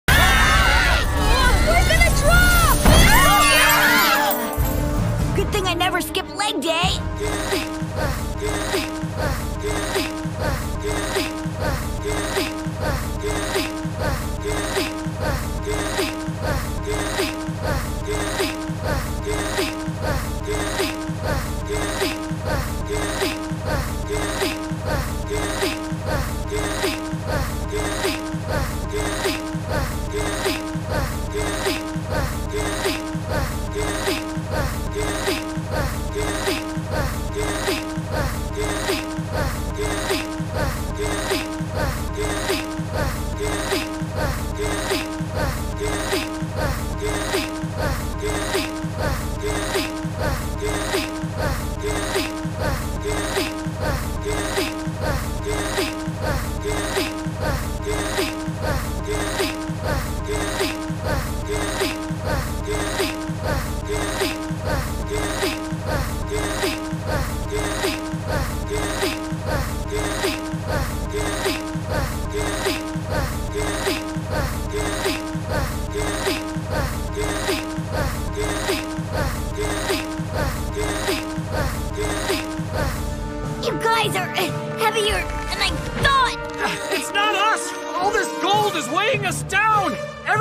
Lynn Jr Hanging On The Cliff While Trying To Lift The Loud Family For 10 Hours!
(Luan Out Loud Reupload)